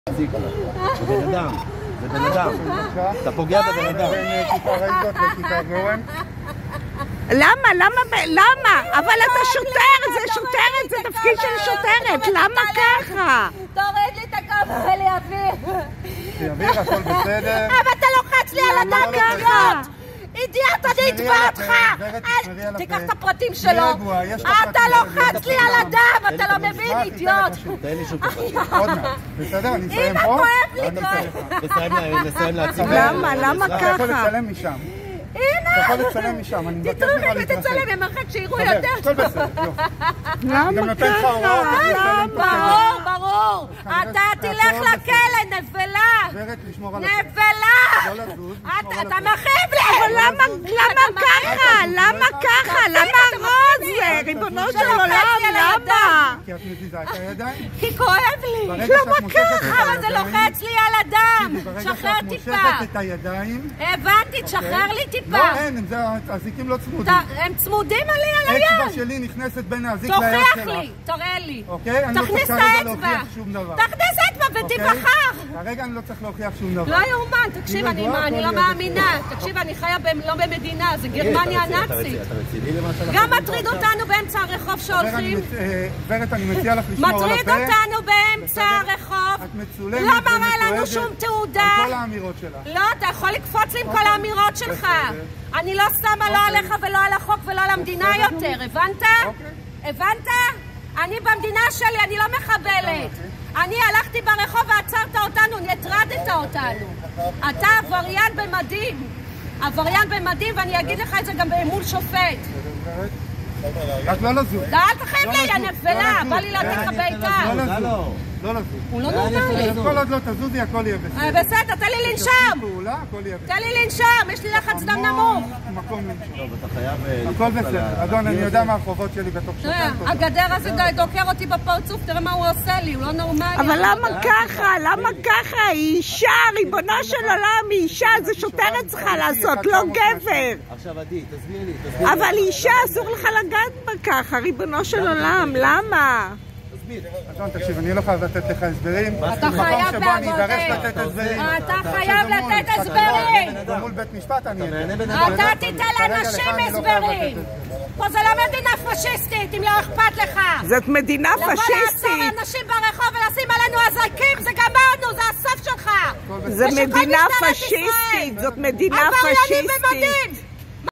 אורי נחמן מפגינה נגד מנדלבליט ונעצרת פרסום לפי סעיף 27 א' לחוק זכויות היוצרים
אורי נחמן מפגינה נגד היועמ"ש מנדלבליט סמוך לביתו בפתח תקווה ונעצרת פרסום לפי סעיף 27 א'  לחוק זכויות היוצרים